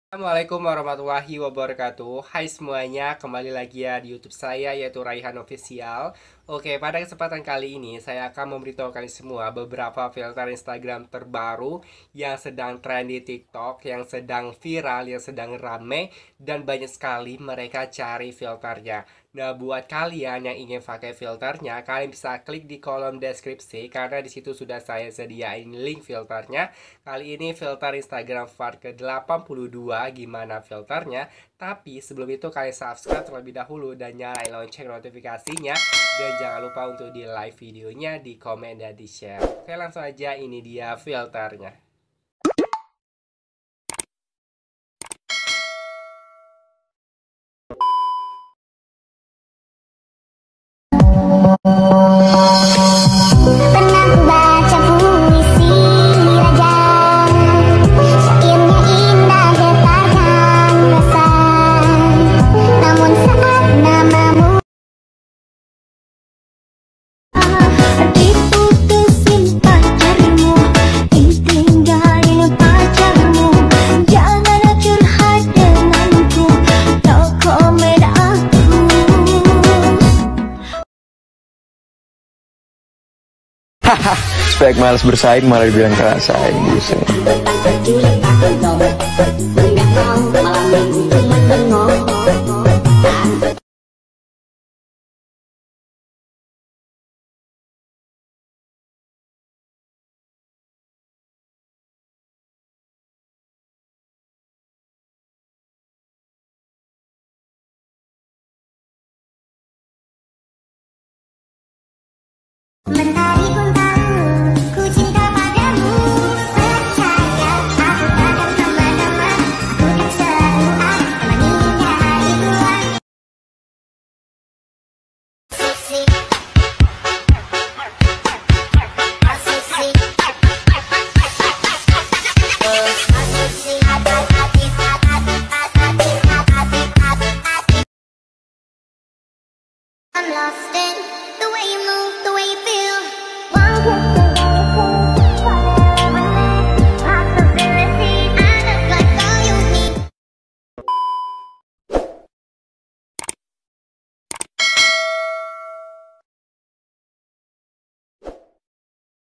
FILTER IG POSISI SHREK VIRAL DI TIKTOK | FILTER IG PART 82
Assalammualaikum
Hi semuanya,kali ini saya memberitahu kalian semua beberapa filter Instagram terbaru yang lagi viral.
Gimana filternya tonton saja ya dan semoga suka.

LINK FILTER

Wassalammualaikum